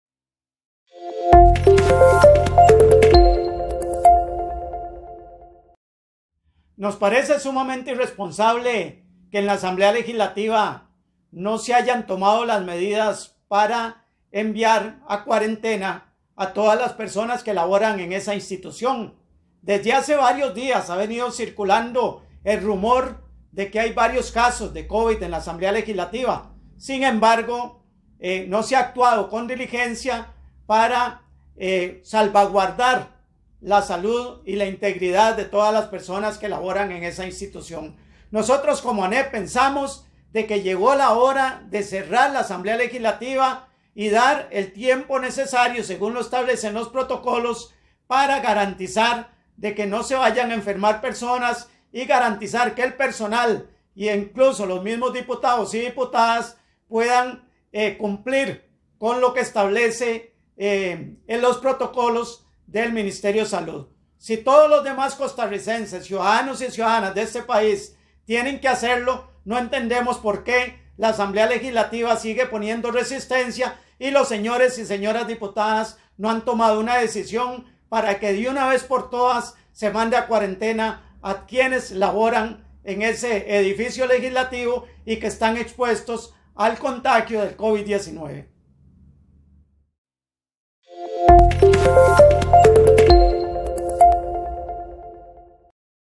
Asamblea Legislativa debe cerrarse como todos los Costarricenses
Un video subido para CRprensa.com exclusivo de noticias en Costa Rica

Este video tiene fines noticiosos, sobre hechos, acontecimientos u opiniones de la realidad nacional en Costa Rica y el mundo.